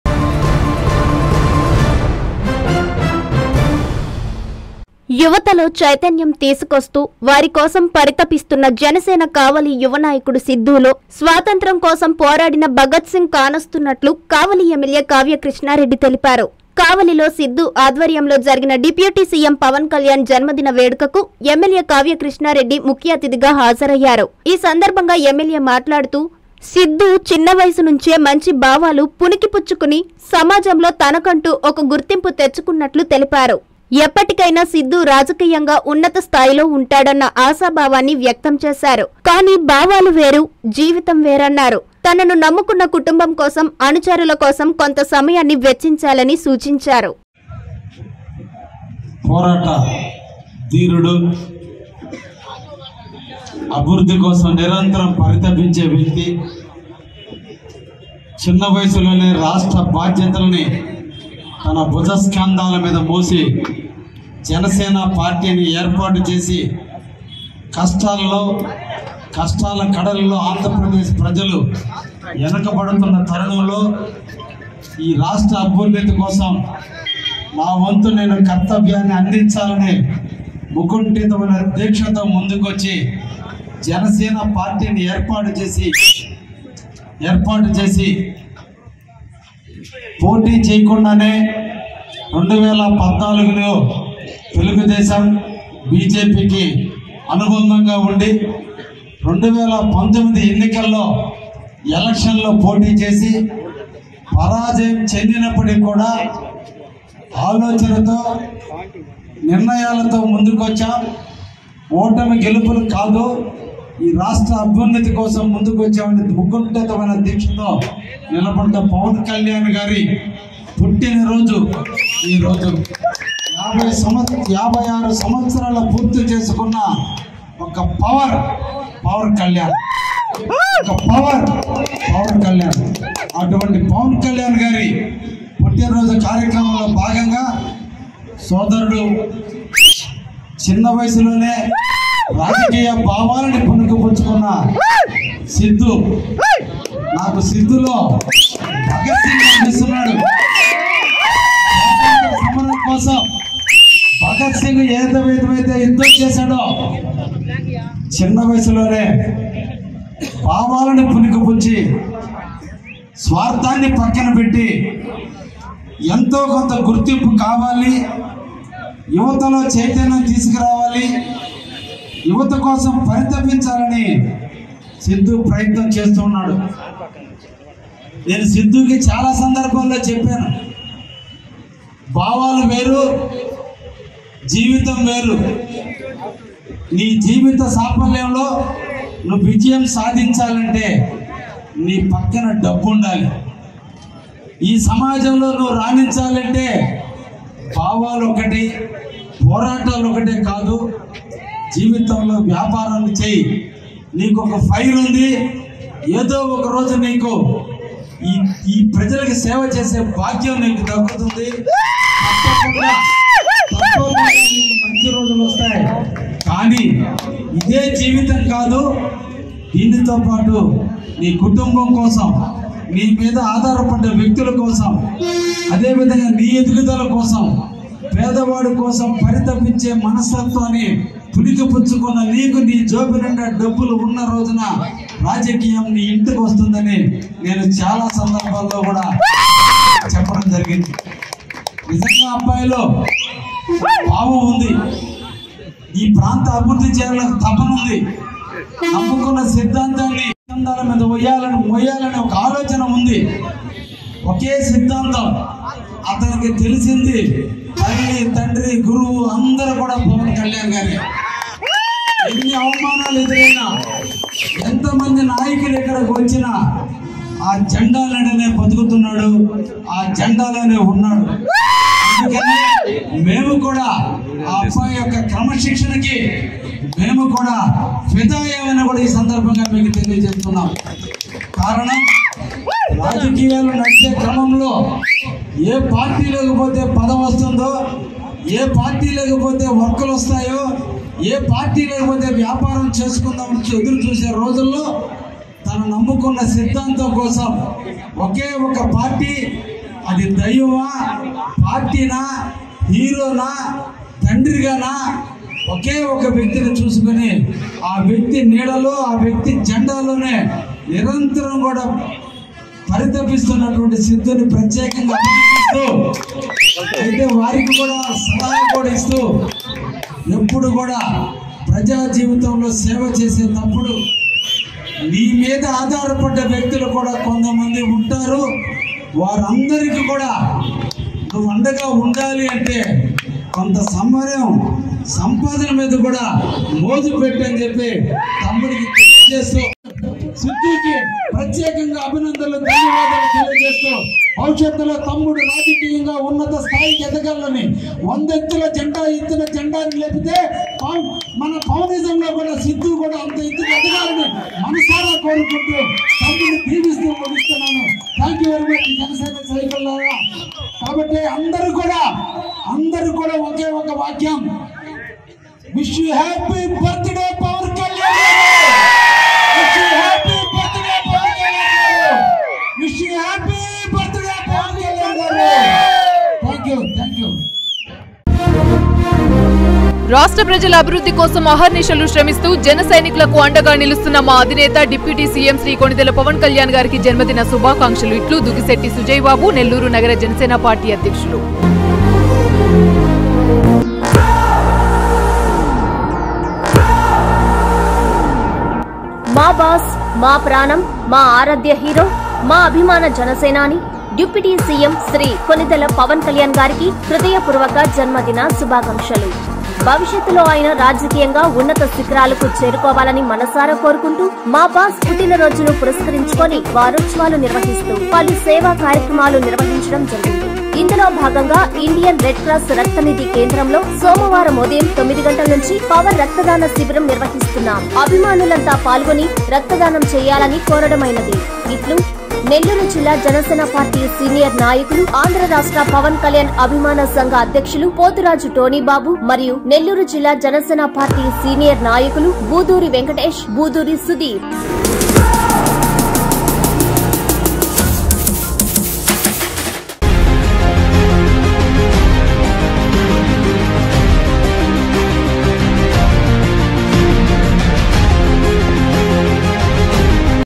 సిద్ధూ క్రమశిక్షణకు ఫిదా అయ్యా...! | MLA Kavya Krishna Reddy praises Janasena Sidhu | Nellore
- జనసేన  సిద్ధూలో ఒక భగత్ సింగ్ ను చూస్తున్నా
- రాజకీయంగా సిద్ధూ ఉన్నత స్థాయికి ఎదిగాలని కోరుకుంటున్నా
- పవన్ కళ్యాణ్ జన్మదిన వేడుకలో కావలి ఎమ్మెల్యే కావ్య కృష్ణారెడ్డి వ్యాఖ్యలిలా